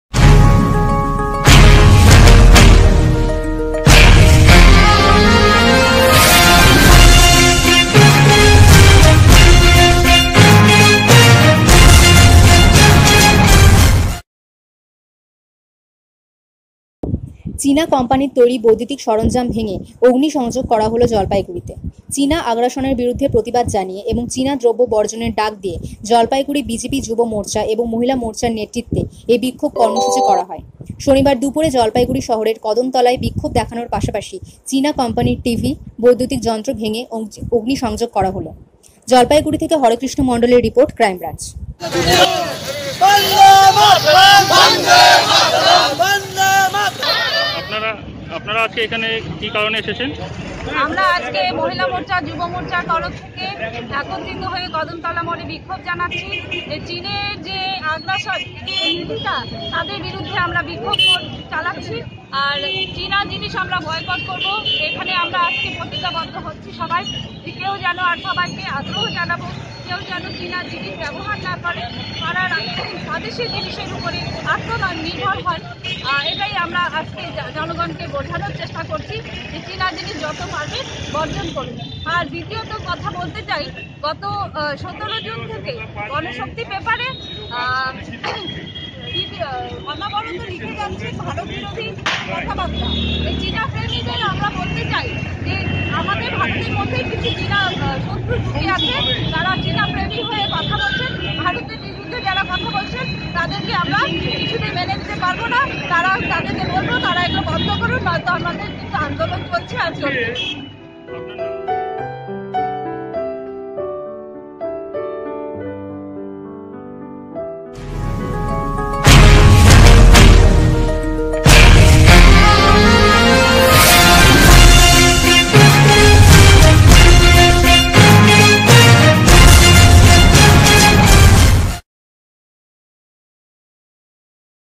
চীনা কোম্পানির তৈরী বৈদ্যুতিক সরঞ্জাম ভেঙে অগ্নিসংযোগ করা হলো জলপাইগুড়িতে
হরেকৃষ্ণ মন্ডল। আলিপুরদুয়ার। ক্রাইম ব্রাঞ্চ।
চীনা কোম্পানির তৈরি বৈদ্যুতিক সরঞ্জাম ভেঙে অগ্নিসংযোগ করা হল জলপাইগুড়িতে। চীনা আগ্রাসনের বিরুদ্ধে প্রতিবাদ জানিয়ে এবং চীনা দ্রব্য বর্জনের ডাক দিয়ে এই বিক্ষোভ কর্মসূচী করে
জলপাইগুড়ি বিজেপি যুব মোর্চা এবং মহিলা মোর্চা নেতৃত্ব।শনিবার দুপুরে জলপাইগুড়ি শহরের কদমতলায় বিক্ষোভ দেখানোর পাশাপাশি চীনা কোম্পানির টিভি, বৈদ্যুতিন যন্ত্র ভেঙ্গে অগ্নিসংযোগ করা হয়।